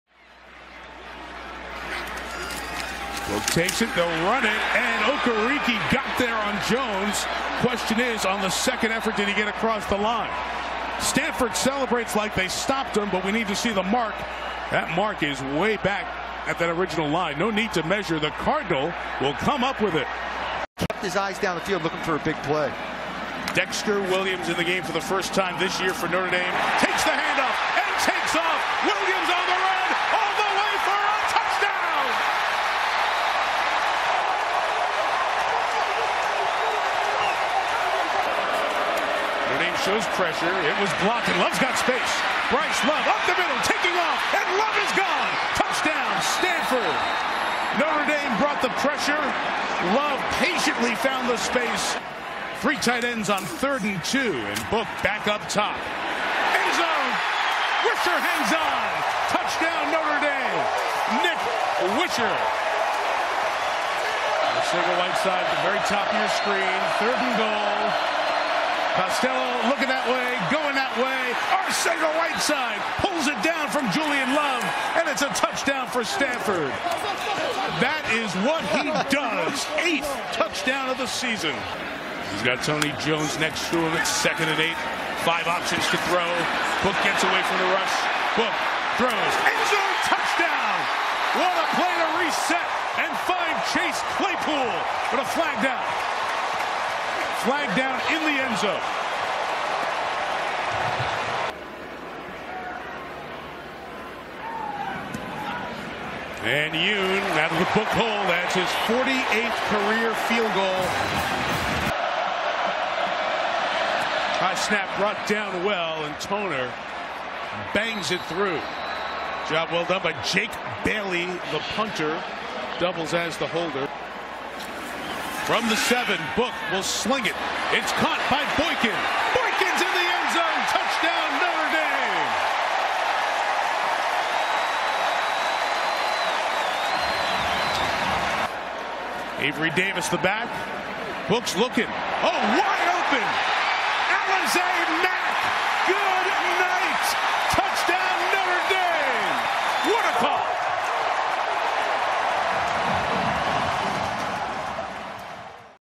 #7 Stanford vs. #8 Notre Dame | 2018 CFB Highlights
#8 Notre Dame dominates #7 Stanford with 38-17 statement win.
Game Information ➤

Final Score ➤ 38-17 Fighting Irish
Line ➤ Notre Dame -5.0
Over/Under ➤ 53
Attendance ➤ 77,622
Location ➤ Notre Dame, IN
Start Time ➤ 7:30 PM EST
Date ➤ September 29, 2018
Coverage ➤ NBC